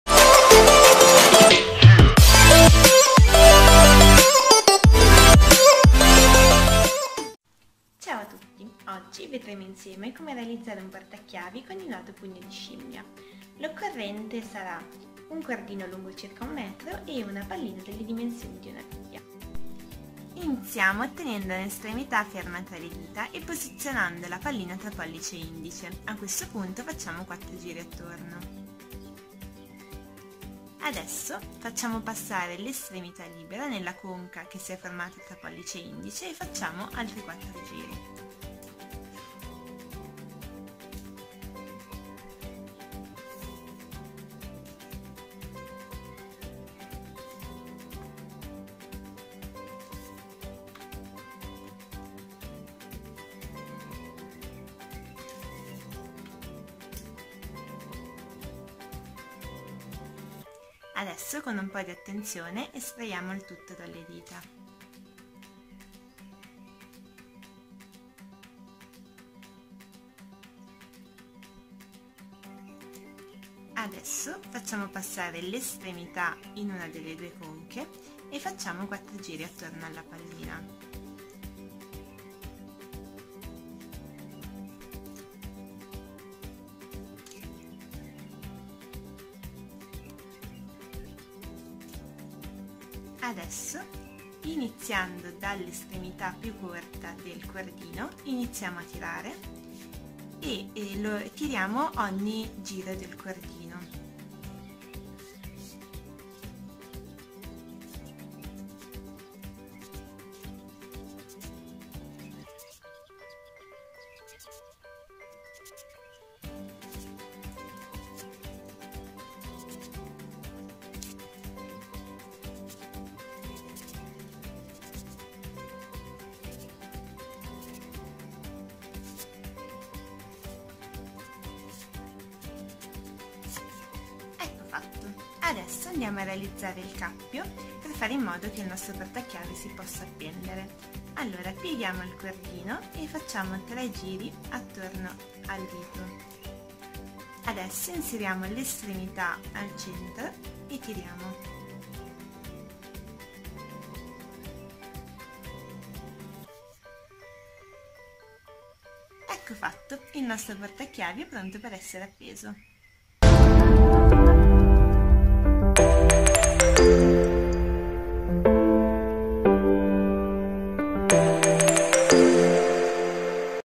Un nodo a pugno di Primate !?!
Il nodo a pugno di scimmia è uno dei nodi più apprezzati al mondo, utilizzato sia a scopo ornamentale, ma anche per la sua utilità nel creare un'estremità pesante che faciliti il lancio di una corda.